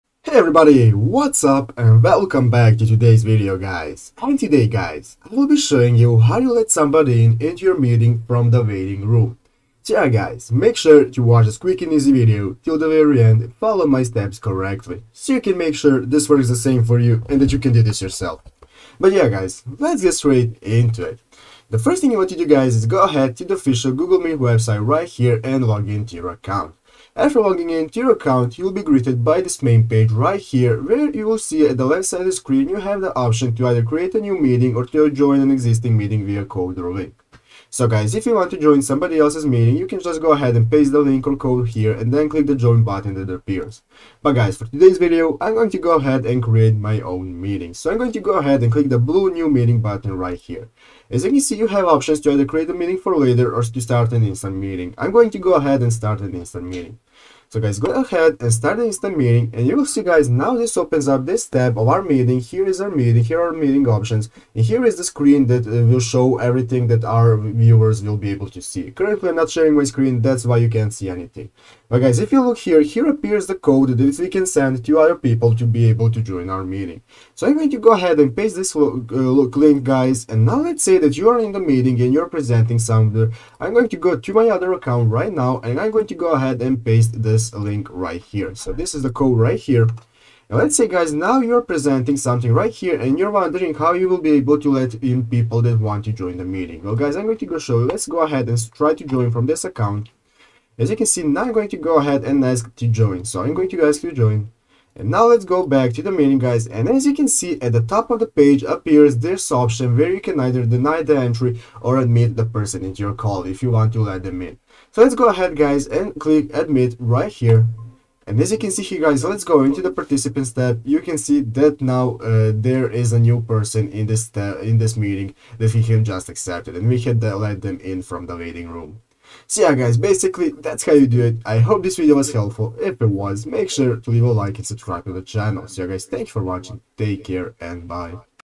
How to Let Someone In Waiting Room Google Meet Tutorial
Today we talk about google meet let someone in waiting room,google meet,waiting room for google meet